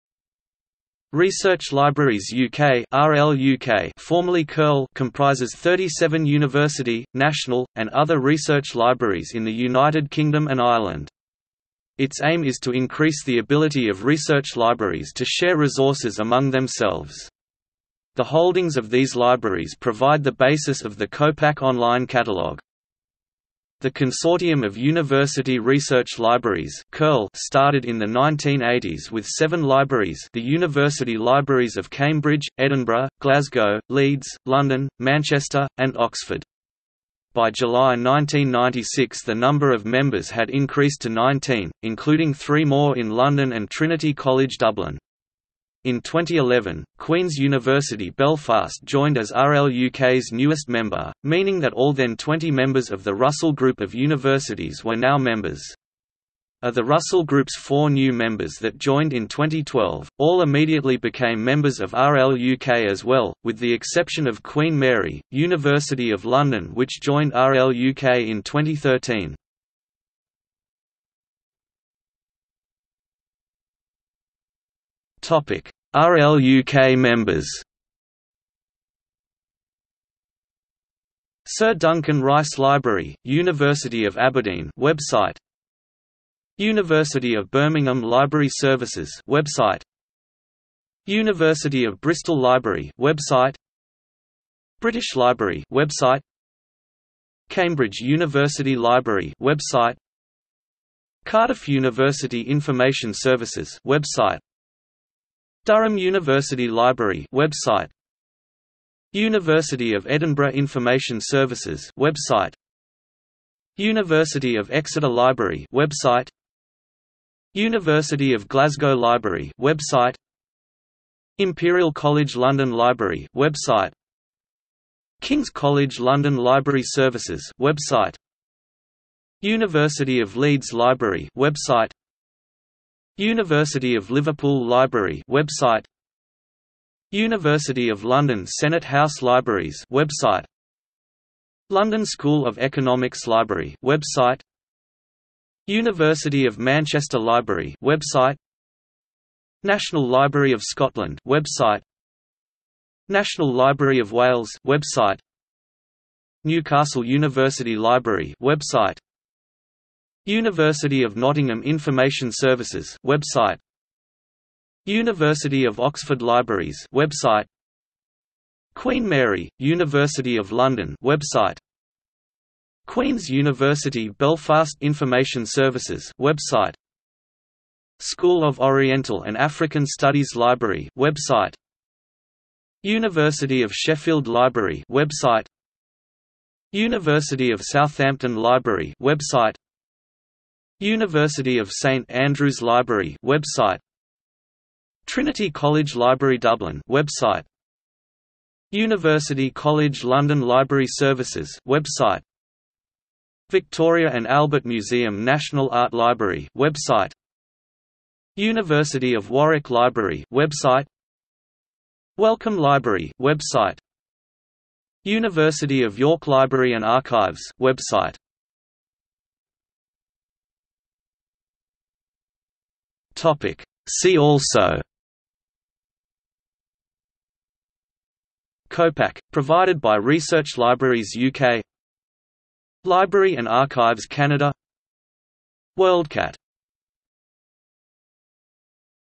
Research Libraries UK | Wikipedia audio article
00:01:11 1 RLUK Members
00:03:39 2 See also

- Socrates

SUMMARY
Research Libraries UK (RLUK) (formerly CURL) comprises 37 University, National, and other research libraries in the United Kingdom and Ireland.  Its aim is to increase the ability of research libraries to share resources among themselves. The holdings of these libraries provide the basis of the Copac online catalogue.
The Consortium of University Research Libraries (CURL) started in the 1980s with seven libraries (the university libraries of Cambridge, Edinburgh, Glasgow, Leeds, London, Manchester, and Oxford).  By July 1996 the number of members had increased to 19, including three more in London and Trinity College Dublin.  In 2011, Queen’s University Belfast joined as RLUK's newest member, meaning that all then-20 members of the Russell Group of universities were now members. Of the Russell Group's four new members that joined in 2012, all immediately became members of RLUK as well, with the exception of Queen Mary, University of London which joined RLUK in 2013.